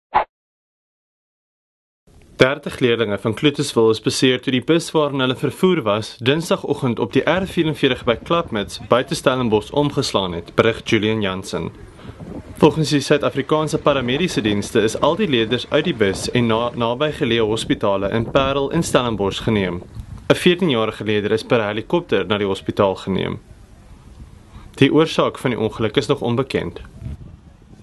Skoolbus-ongeluk buite Stellenbosch
30 leerlinge van Cloetesville is beseer toe die bus waarin hulle vervoer was, Dinsdagoggend op die R44 by Klapmuts buite Stellenbosch omgeslaan het berig Julian Jansen.